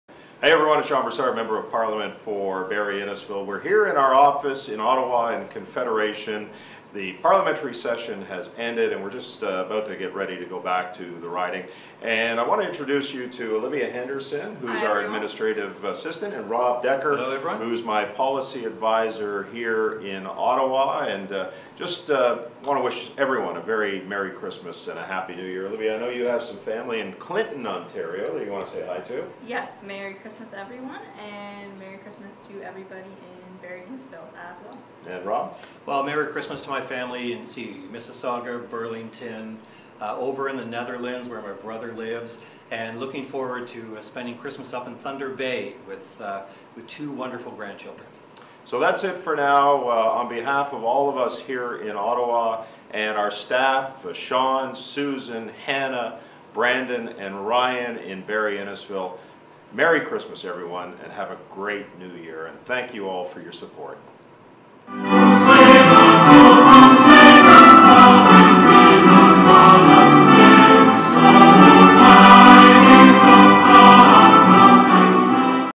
Christmas Greetings from Ottawa and Barrie-Innisfil
Merry Christmas from our Barrie-Innisfil and Ottawa office! Wishing you and yours a happy and safe holiday season and prosperous New Year!